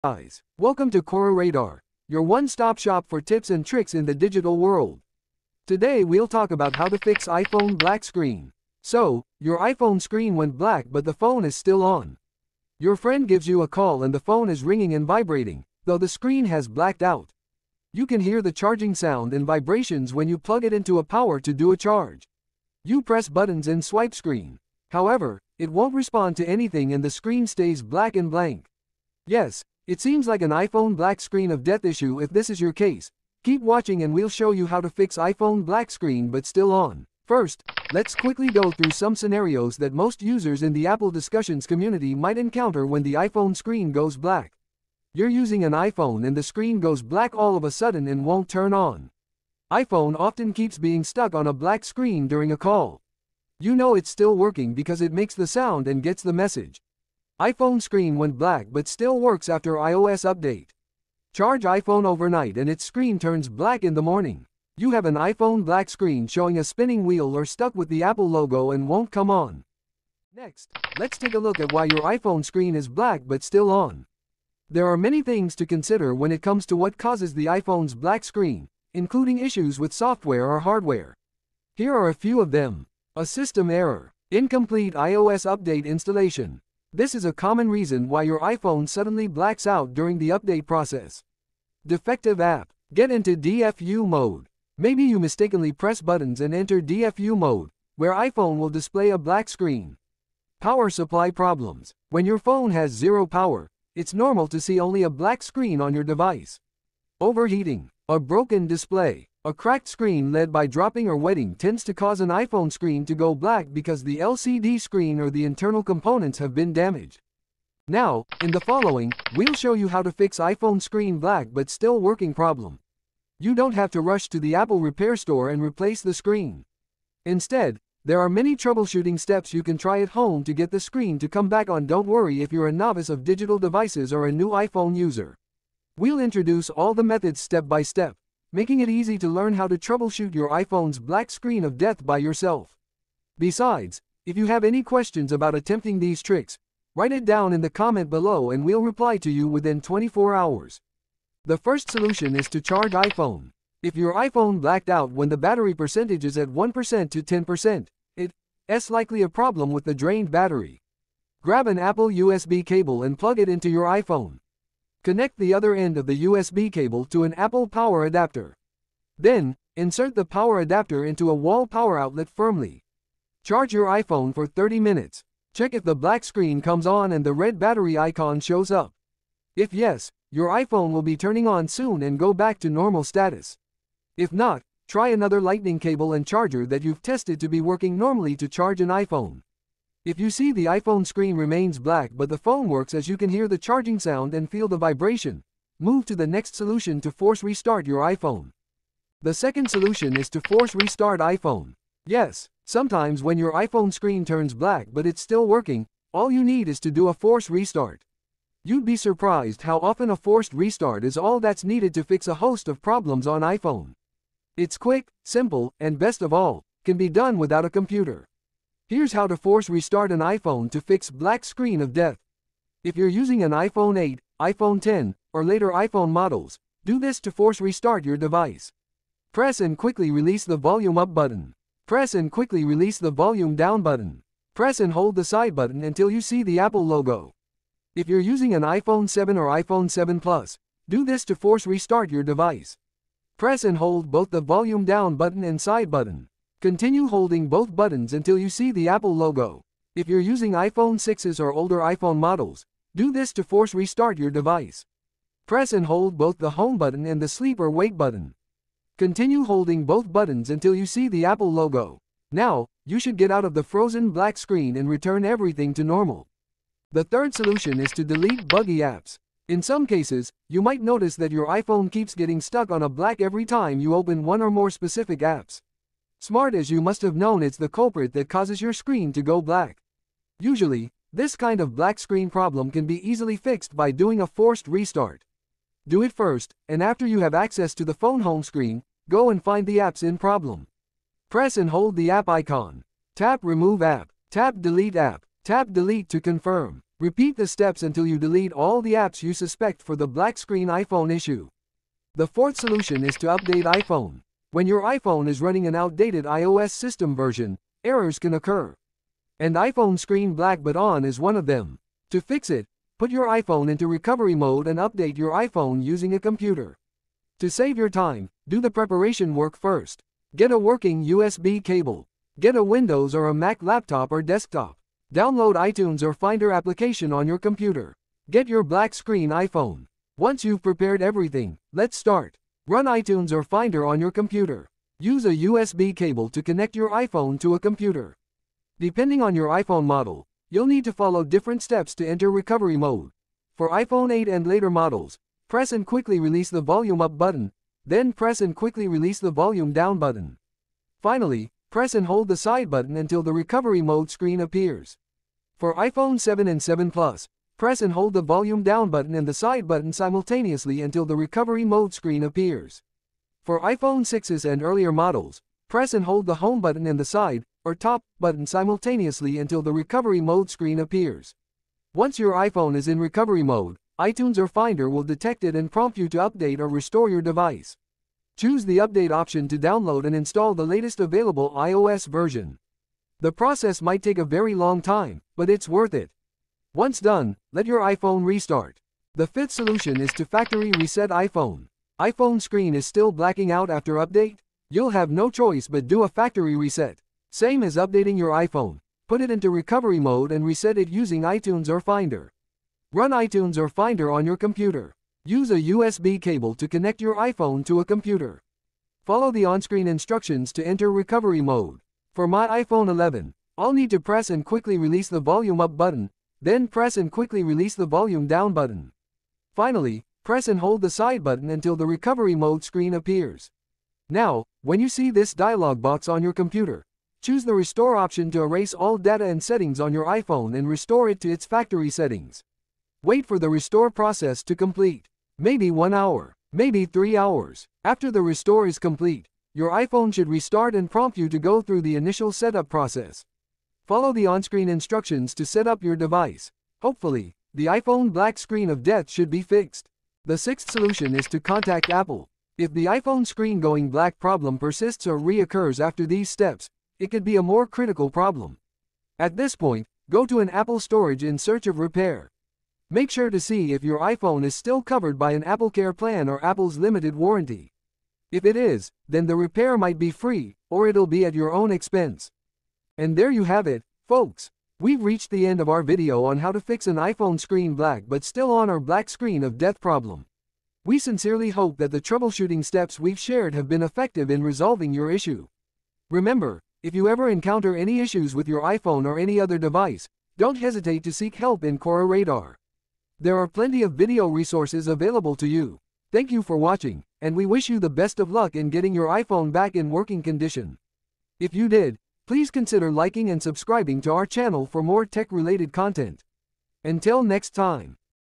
iPhone Screen Black But Still Working? Here’s How to Fix iPhone Black Screen of Death Issues
Today we’ll talk about how to fix iPhone black screen. So, your iPhone screen went black but the phone is still on. Your friend gives you a call and the phone is ringing and vibrating, though the screen has blacked out. You can hear the charging sound and vibrations when you plug it into a power to do a charge. You press buttons and swipe screen. However, it won’t respond to anything and the screen stays black and blank. Yes, it seems like an iPhone black screen of death issue. If this is your case, keep watching and we’ll show you how to fix iPhone black screen but still on.

0:00 Background
0:37 Some iPhone black screen scenarios
1:10 Why your iPhone screen is black but still on
1:57 How to fix iPhone screen black but still working
2:31 1️⃣Charge iPhone
3:19 2️⃣Force restart iPhone
4:26 3️⃣Delete buggy apps
5:02 4️⃣Update iPhone
6:29 5️⃣Factory reset iPhone
7:30 6️⃣Contact Apple